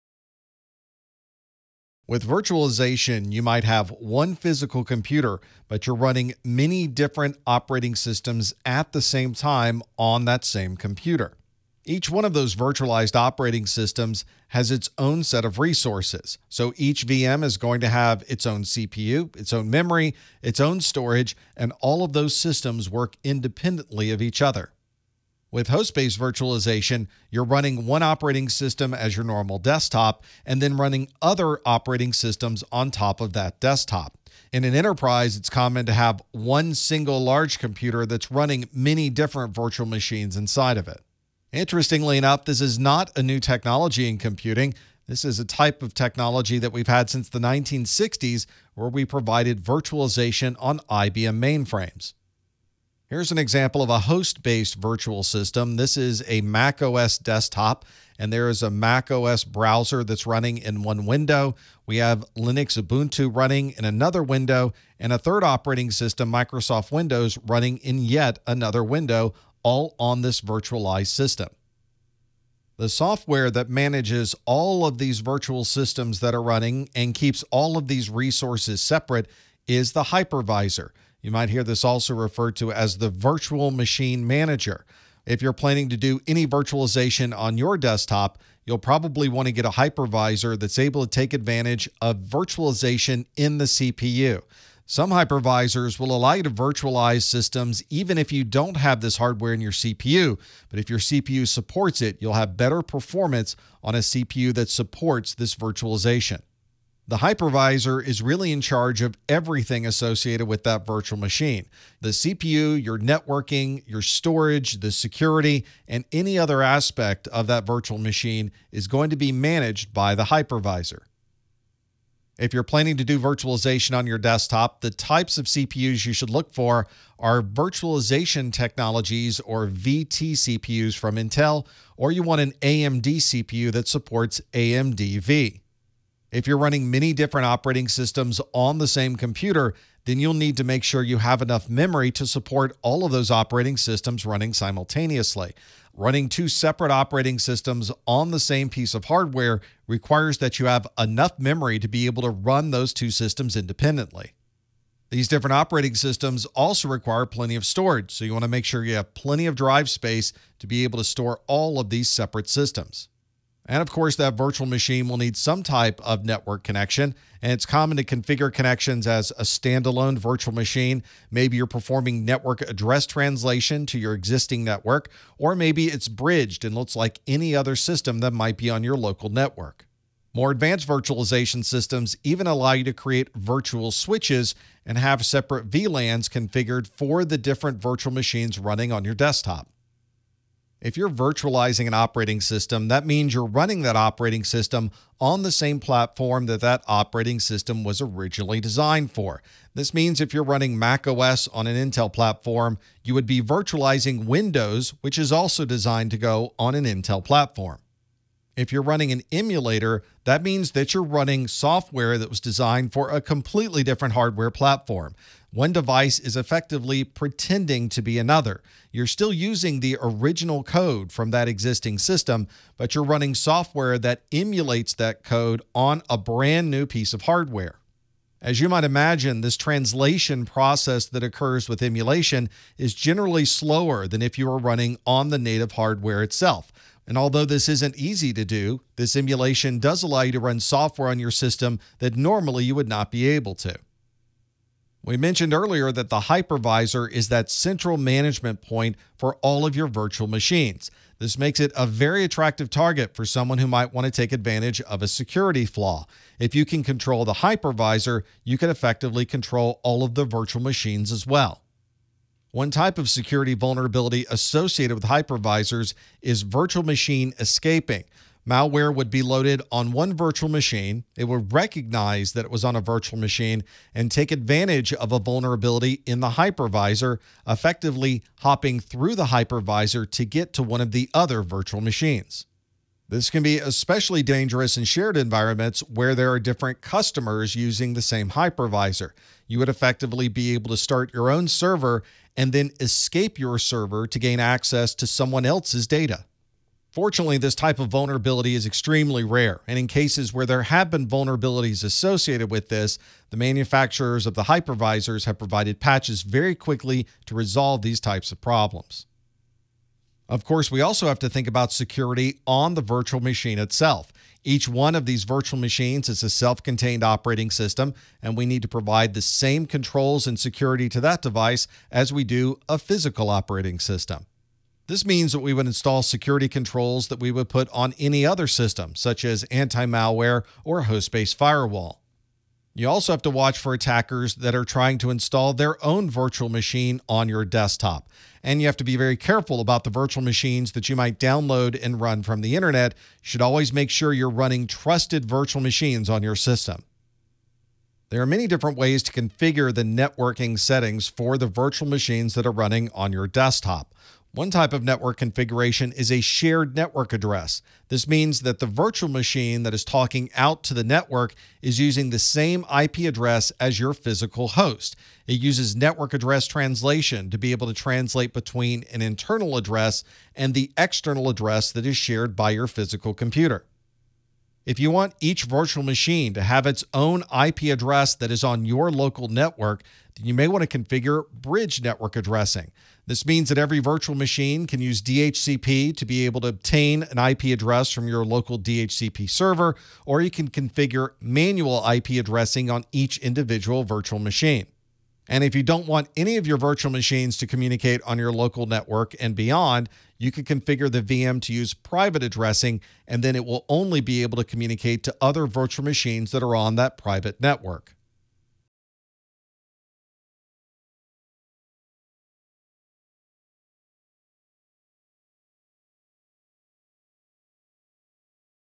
Client-side Virtualization - CompTIA A+ 220-1001 - 4.2
Client-side virtualization provides a way to run many different operating systems on the same desktop. In this video, you’ll learn about the resource requirements and network configurations for client-side virtualization.